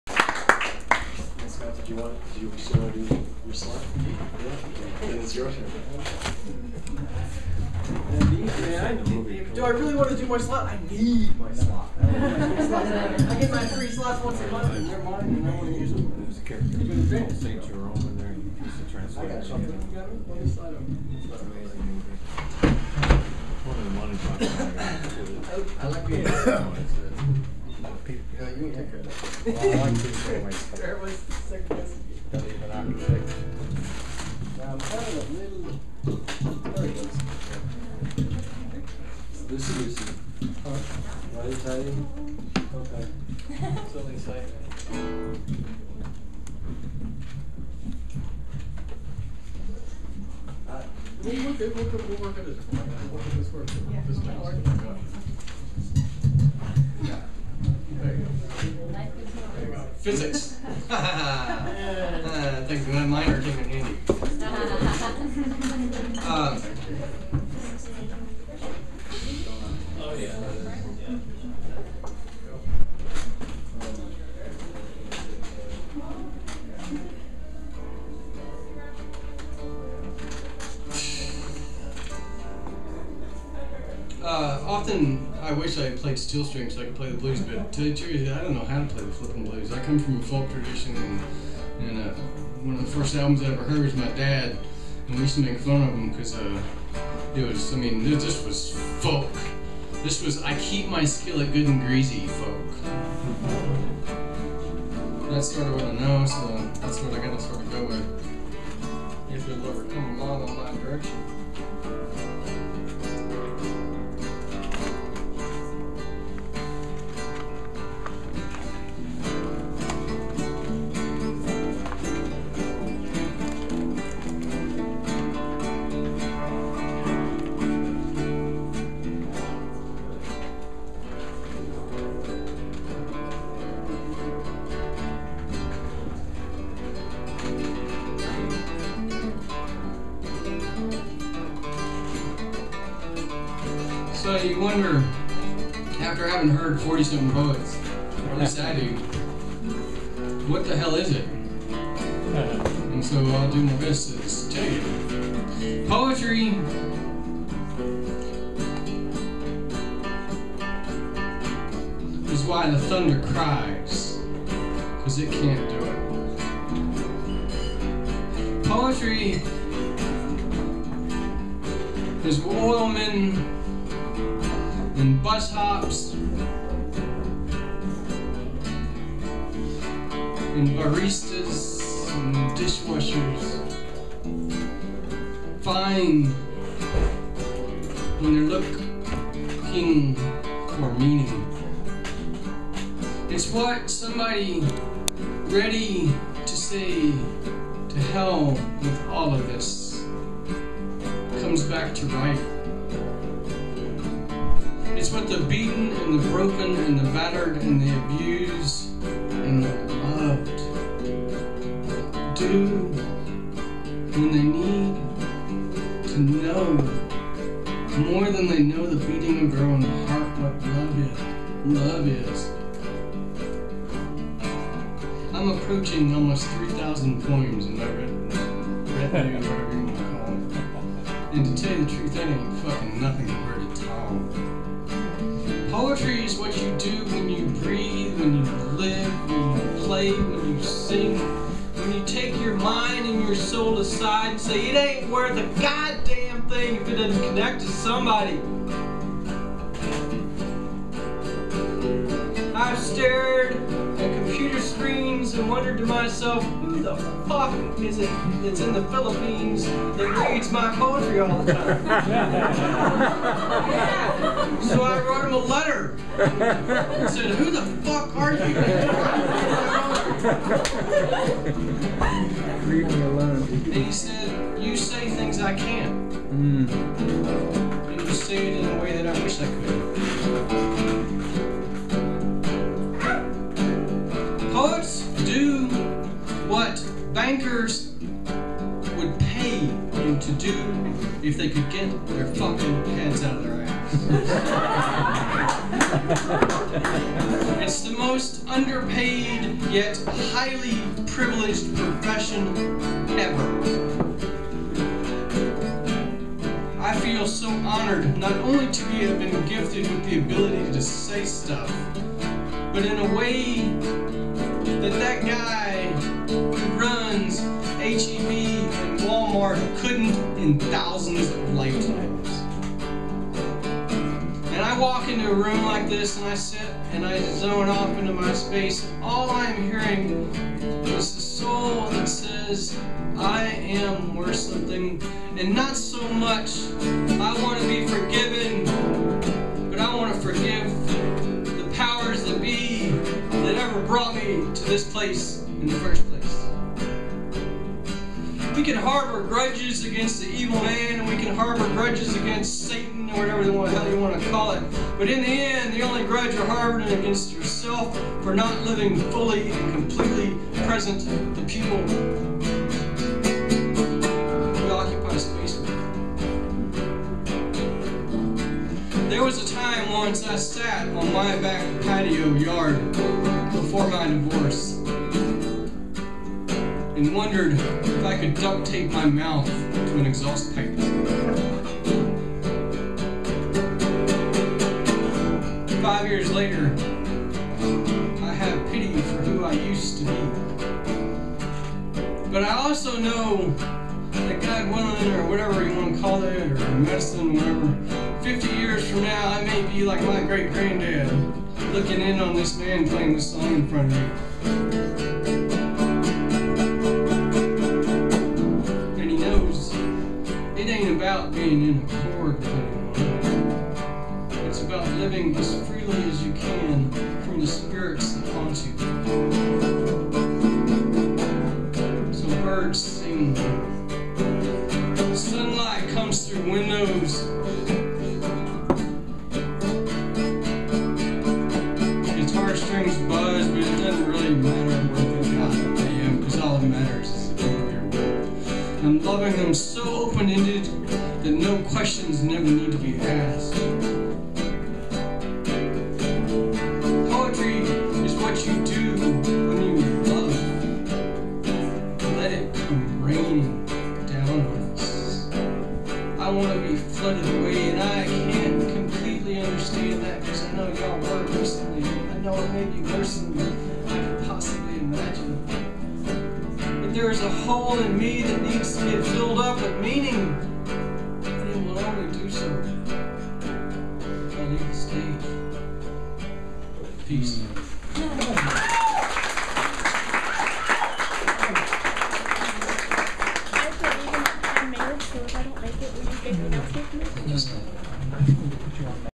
2015 11 06 01:52:47 - Why The Thunder Cries / We Wonder
Date: Thursday, November 5, 2015
Event: San Marcos Poetry Night
Location: Wake the Dead Coffee House, San Marcos, Texas, USA